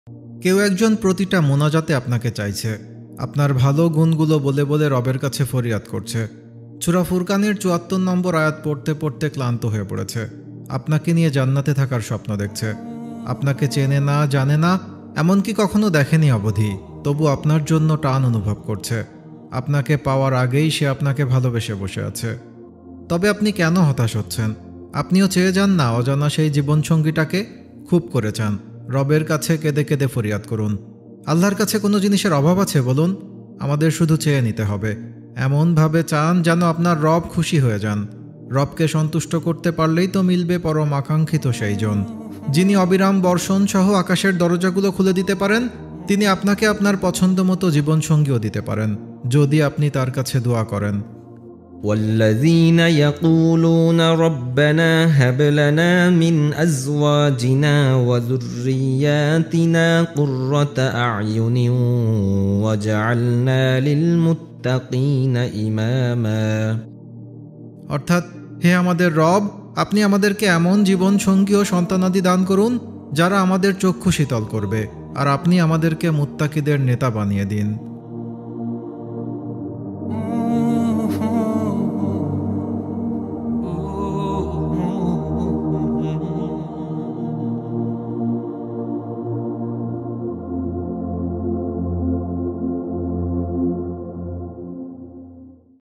উত্তম জীবনসঙ্গী পাওয়ার দোয়া | Deen Daily
আপনি কেনো হতাশ হচ্ছেন? আল্লাহর কাছে কোনো জিনিসের অভাব আছে বলুন? আমাদের শুধু চেয়ে নিতে হবে। এমনভাবে চান যেন আপনার রব খুশি হয়ে যান। রবকে সন্তুষ্ট করতে পারলেই তো মিলবে পরম আকাঙ্ক্ষিত সেই জন!
যিনি অবিরাম বর্ষণসহ আকাশের দরজাগুলো খুলে দিতে পারেন, তিনি আপনাকে আপনার পছন্দমত জীবনসঙ্গীও দিতে পারেন। যদি আপনি তাঁর কাছে দুআ করেন।